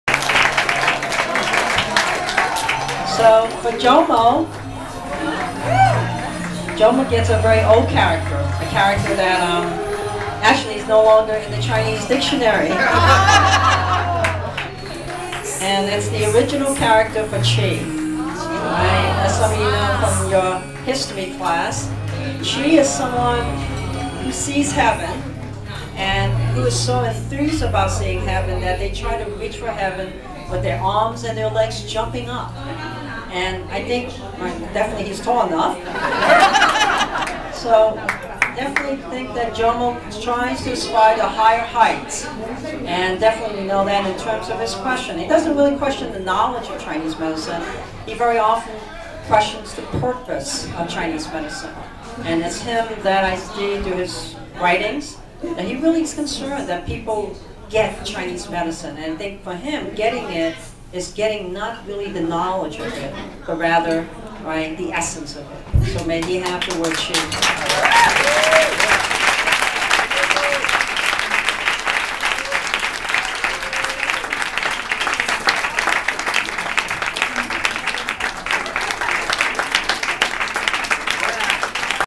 Qi - Jomo
Jomo receives the ancient character for qi from Jeffrey.